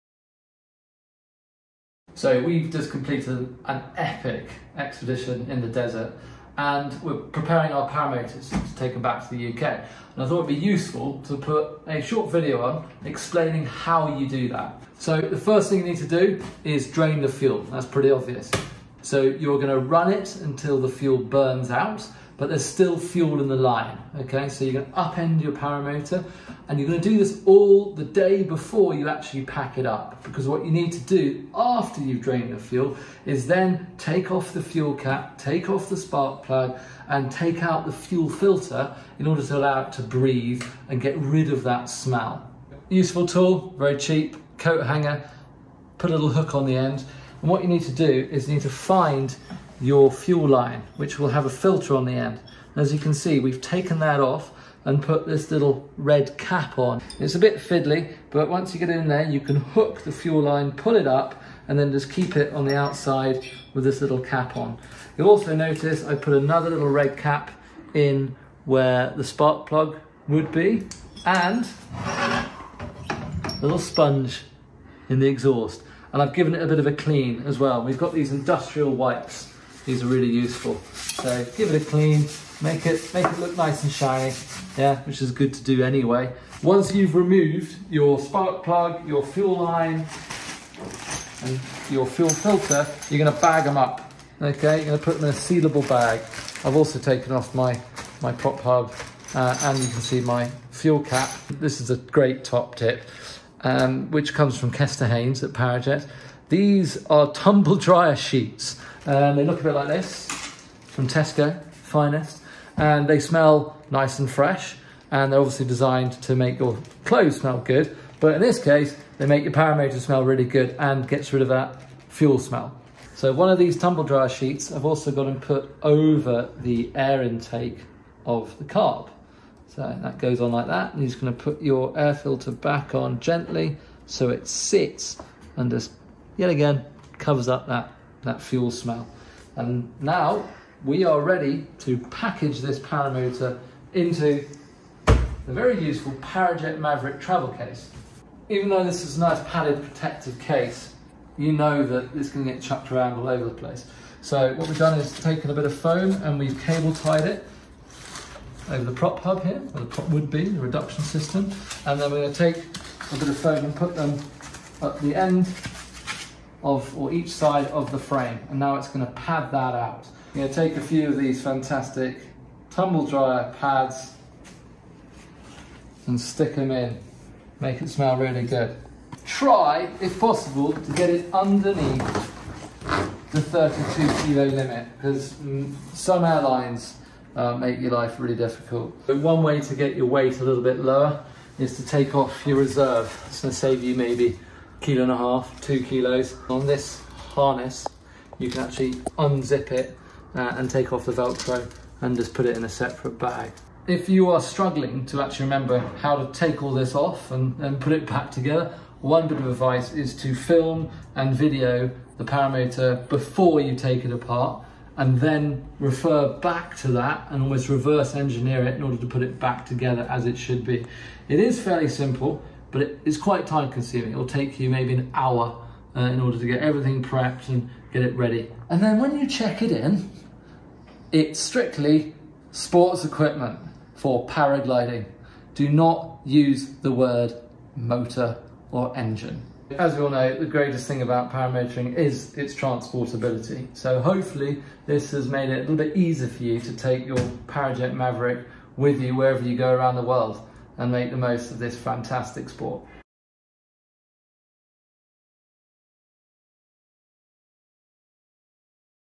How to prepare your Parajet Maverick for Travel
One of the great things about the Parajet Maverick is it's transportability. This film explains how to prepare your Paramotor for travel so you can take it with you and make the most of this fantastic sport around the world. See below for a step by step process:

The day before you plan to travel:

1. Drain all fuel, run the engine until it dies, disconnect the fuel line and clean the engine
2. Remove the fuel filter from the fuel tank using a coat hanger to hook the fuel line
3. Remove the spark plug from the cylinder head
4. Remove the air filter from the carburettor
5. Remove the fuel cap from the fuel tank
6. Allow the engine to ventilate and any remaining fuel to evaporate over night

The day you plan to travel:

1. Plug and seal the cylinder head, fuel line and exhaust outlet
2. Place a tumble dryer sheet between the air filter and carburettor
3. Place spark plug, fuel line and fuel filter in a sealed bag and transport them separately
4. Remove the reserve parachute to reduce weight and transport it separately
5. Securely protect the Paramotor in the travel case and add some tumble dryer sheets.

Don't forget to check the airlines rules and regulations. Most airlines allow you to travel with up to 32kgs if you pay for excess luggage. The US can be very difficult due to the TSA's strict rules so consider shipping with UPS instead. If travelling with an electric start do not travel with the battery. When checking in mention that it's Sports Equipment for Paragliding!